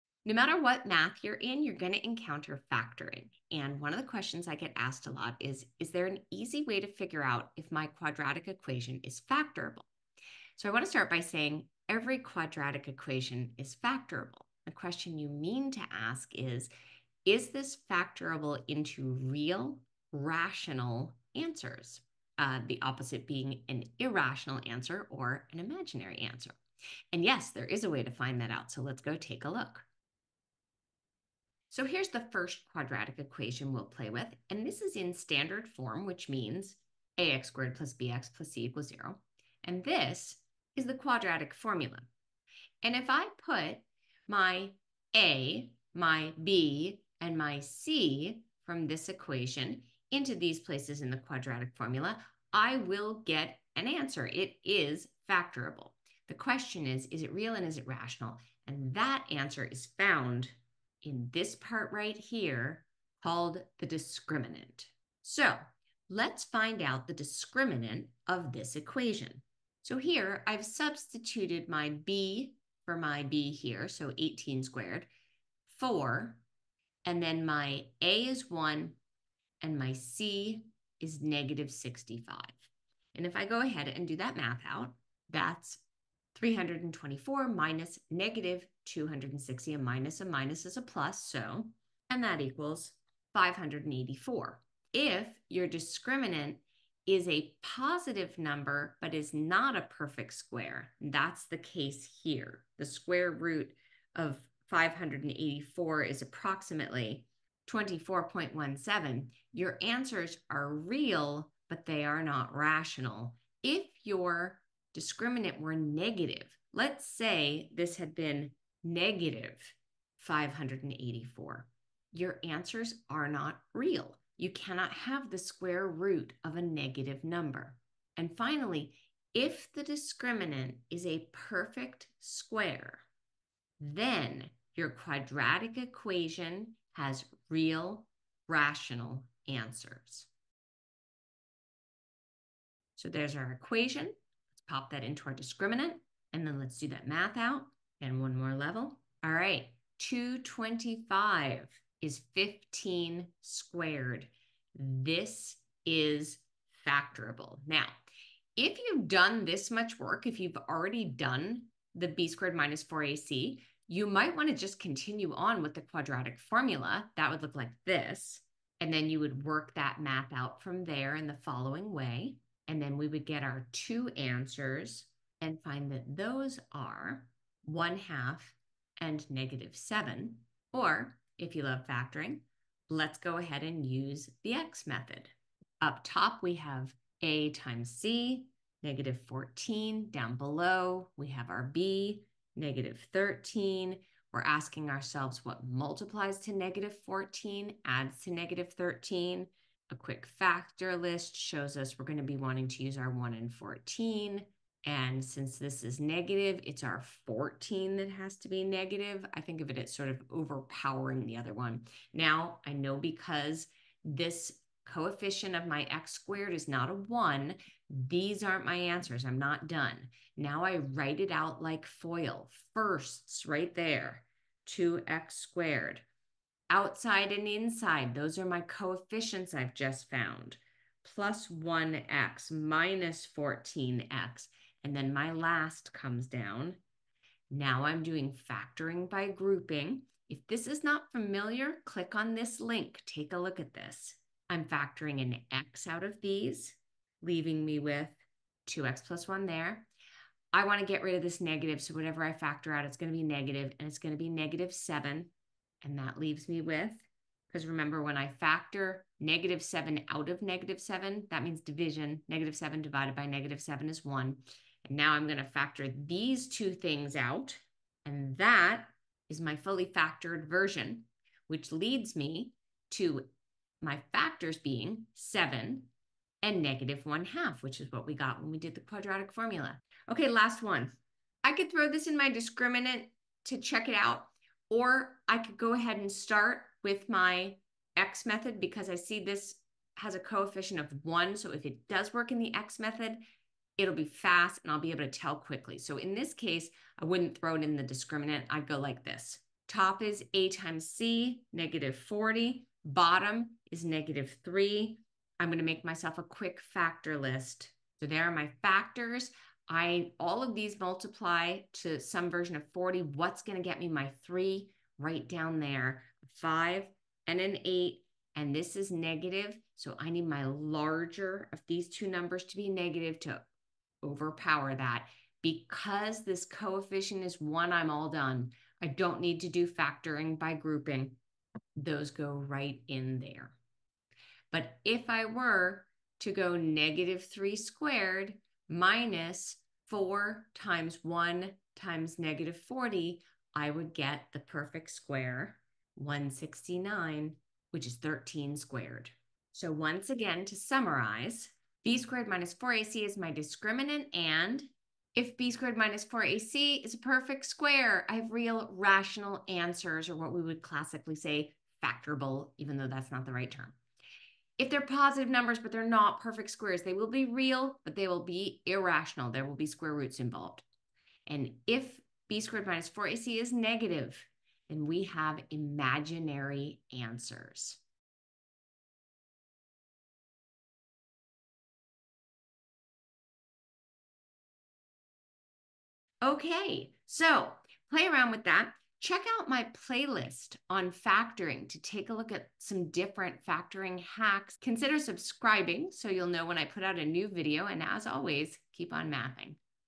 Is My Quadratic Factorable?
Actually, every quadratic is factorable, but you can use a portion of the quadratic formula called the Discriminant to determine whether your quadratic has real and rational solutions, real and irrational solutions or imaginary solutions, and how many!  Also covers factoring quadratic equations using the X method, as well as factoring by grouping and using the Quadratic Formula.